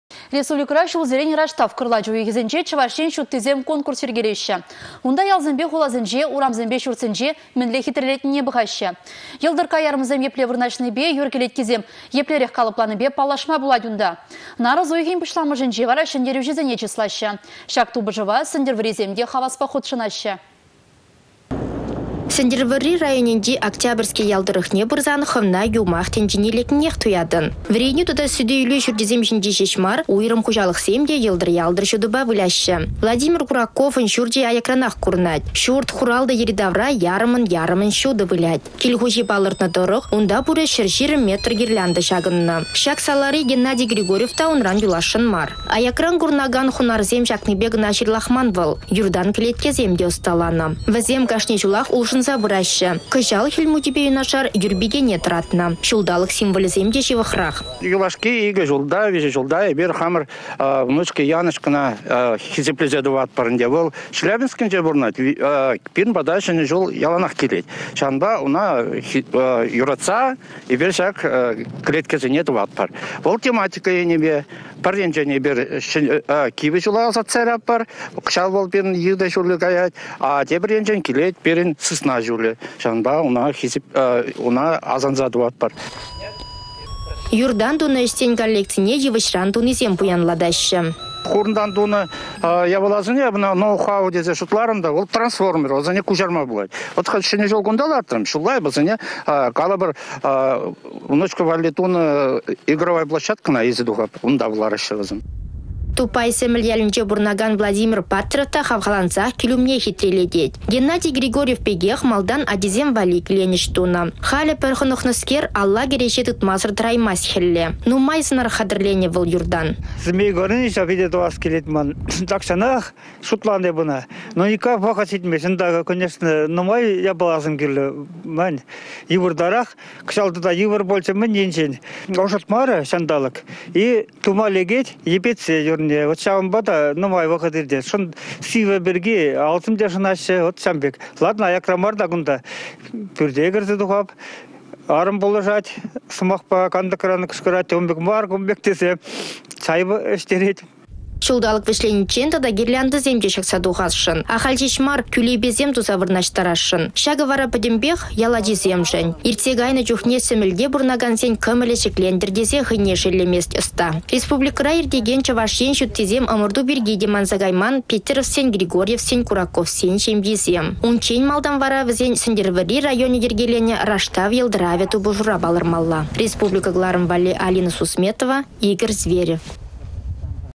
Раштав йăлтăравне хатĕрленеççĕ
Республикăра çулсерен раштав-кăрлач уйăхĕсенче «Чăваш Ен çуттисем» конкурс йĕркелеççĕ. Унта ялсемпе хуласенче урамсемпе çуртсене мĕнле хитрелетнине пăхаççĕ. Йăлтăркка ярăмсем епле вырнаçнипе, юр кĕлеткисем еплерех калăпланипе паллашма пулать унта. Нарăс уйăхĕн пуçламăшĕнче вара çĕнтерӳçĕсене чыслаççĕ. Çак тупăшăва сĕнтĕрвăррисем те хаваспах хутшăнаççĕ.